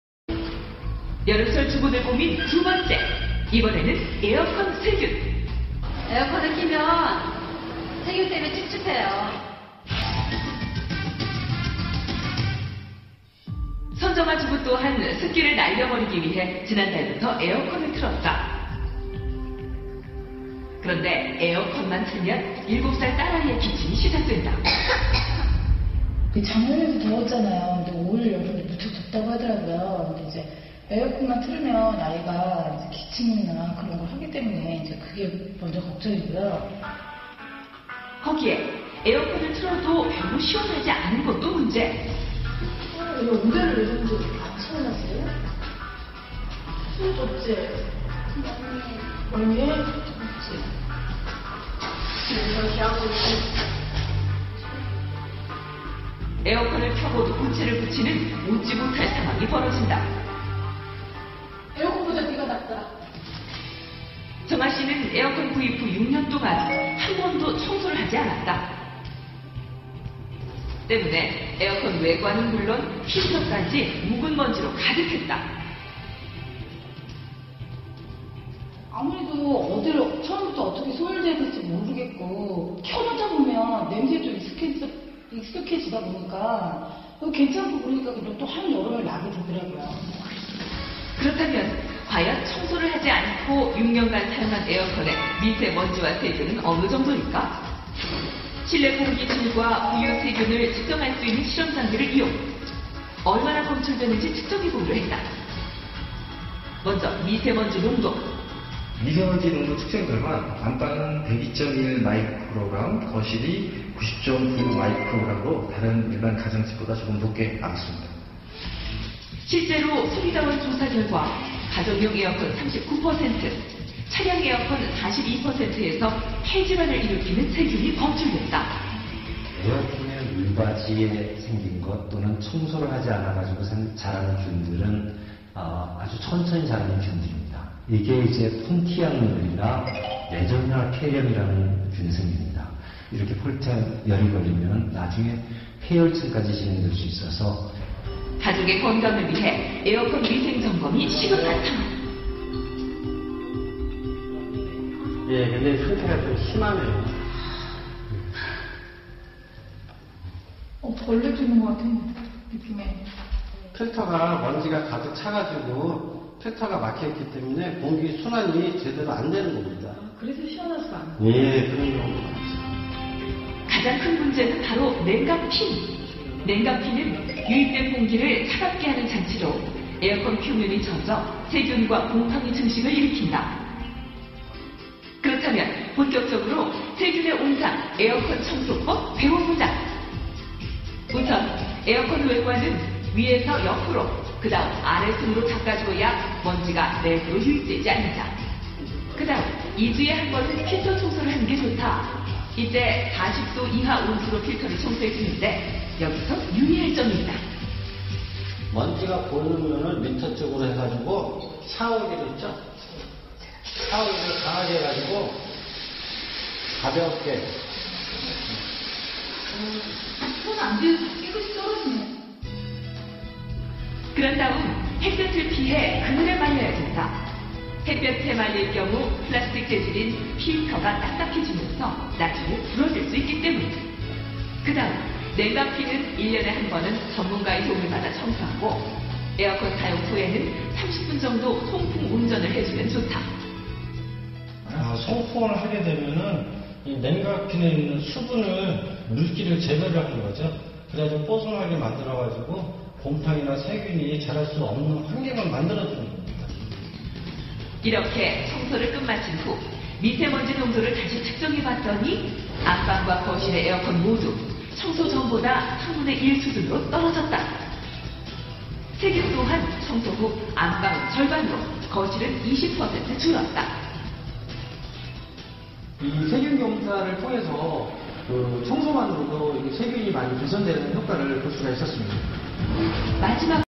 청소곰 건강정보 에어컨청소방법
에어컨 청소방법

(1) 에어컨 외관은 위→옆→아래의 순서로
(2) 필터청소는 2주에 1번 온수로 먼지제거
(3) 온수로 세척한 필터는 그늘에 말린다
(4) 냉각핀은 1년에 1번 전문가에게 의뢰하여 청소한다
(5) 에어컨 사용 후에는 30분정도 송풍운전을 실시하여
    냉각핀을 건조시키는 것이 좋다